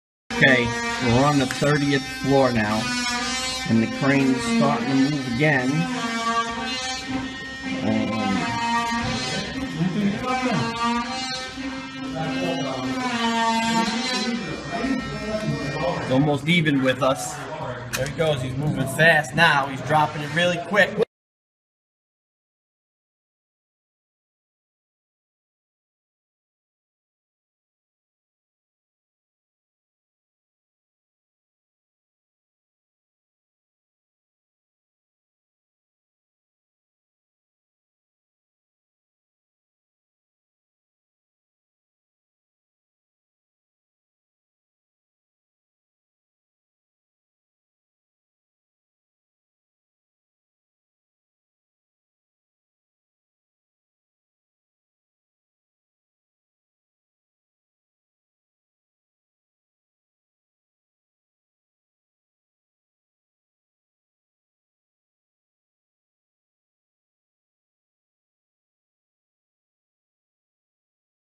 Onlooker films New York crane collapse
A large construction crane has collapsed in lower Manhattan in New York City, killing one person and injuring at least two others.
The collapse happened along West Broadway early on Friday morning in the Tribeca neighbourhood.
Glenn Zito a construction worker captured the moment of collapse on his mobile phone.